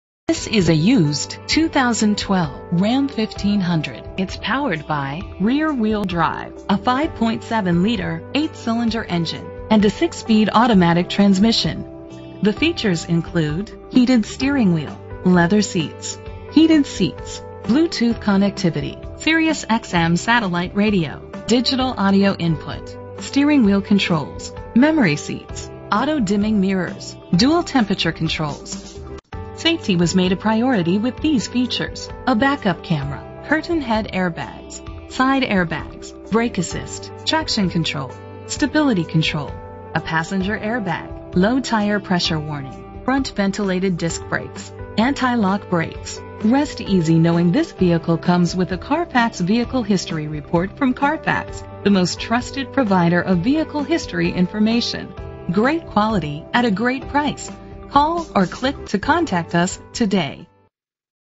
2012 RAM 1500 - Pensacola FL
Year: 2012
Make: RAM
Model: 1500
Trim: Laramie
Engine: 5.7 liter 8 cylinder 16 valve
Transmission: 6-Speed Automatic
Color: Black
Mileage: 47096
Address:
230 Beverly Parkway